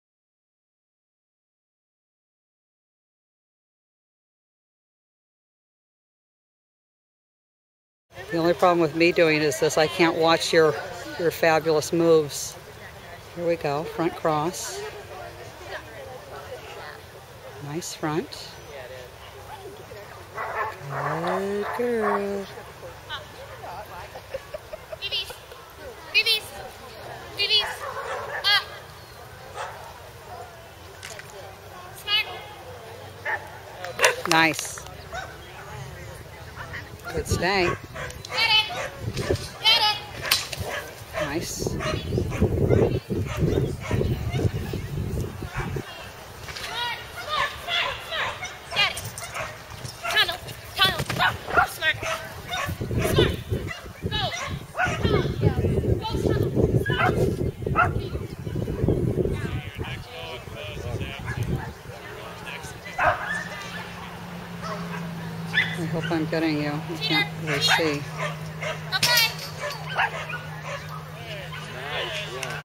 07/22/2012: Smart, AKC, Excellent B Standard & Jumpers 16", 1st places, QQ #8
07/22/2012: Smart, AKC, Excellent B Standard & Jumpers 16", 1st places, QQ #8

Wow! The baby is really growing up! QQ #8! I can't believe it.

Smart turned 2 1/2 on 6/30/2012!